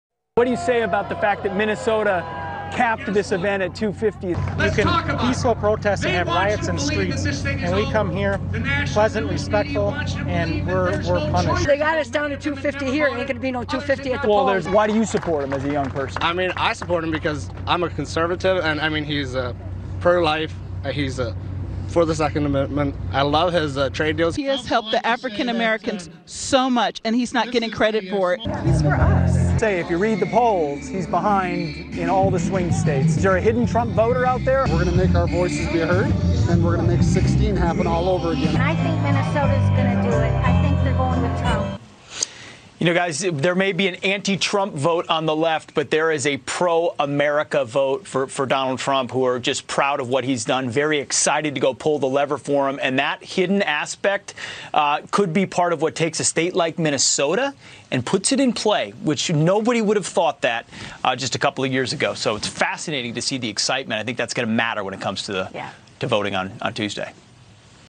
Thousands of Trump Supporters Were Prevented from Entering a Minnesota Rally
The Democrat governor of Minnesota denied thousands of Trump supporters the right to hear their President yesterday, but they will make their voices heard on Election Day!

“They got us down to 250 here, ain’t gonna be no 250 at the polls.”